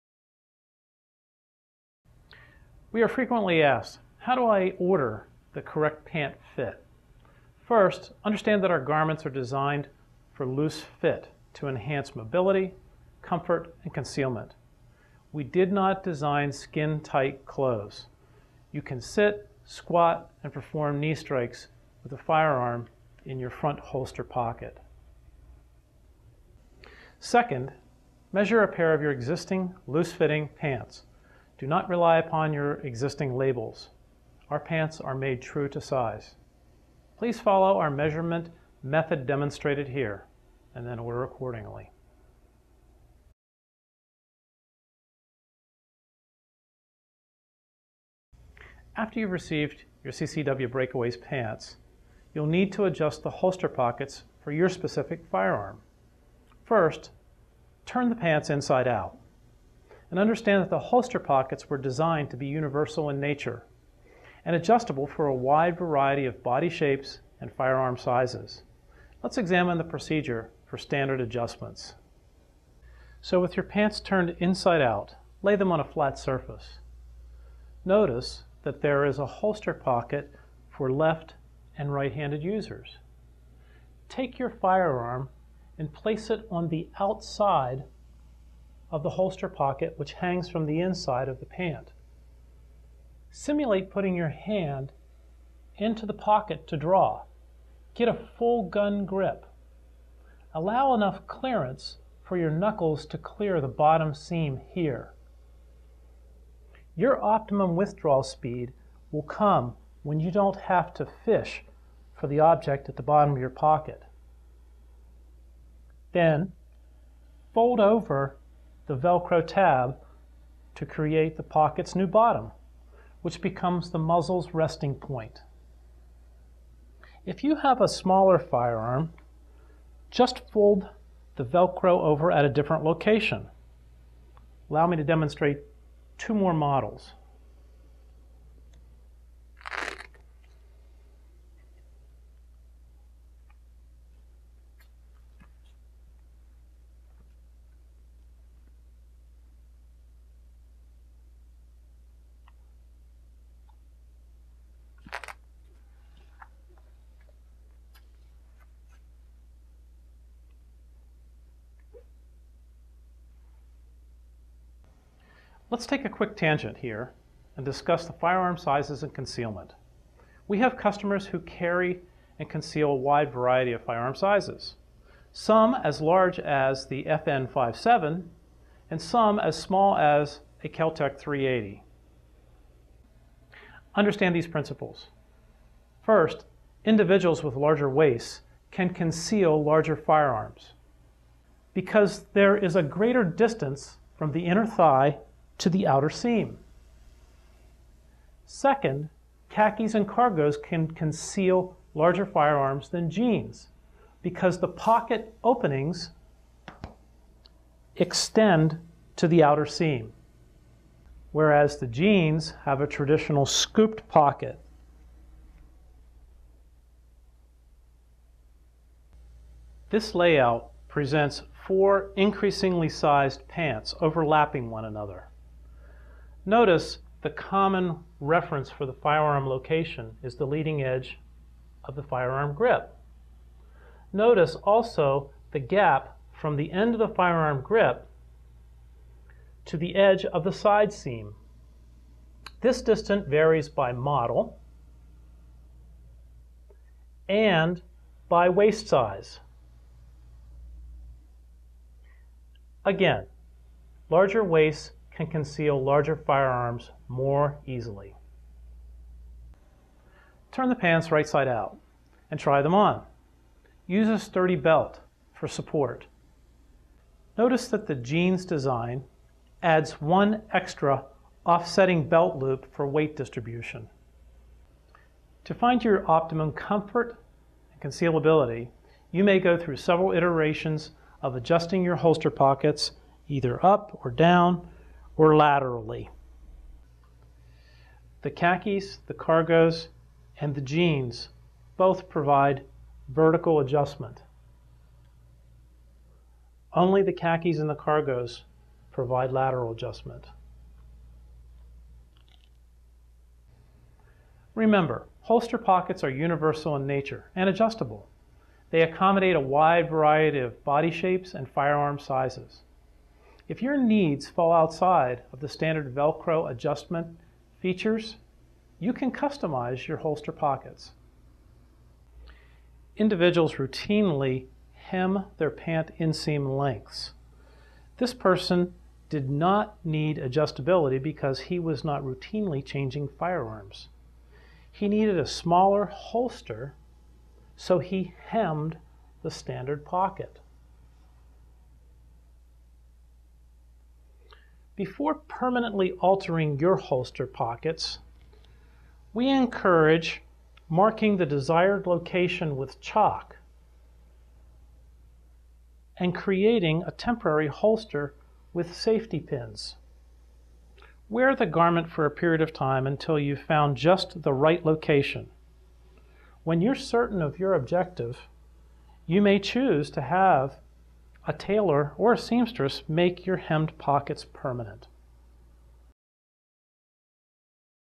Proper Fit & Adjustment
This video will help you understand how to order the correct pant size and to get the proper fit.
You will then learn how to properly adjust the holster-pockets for your specific firearm.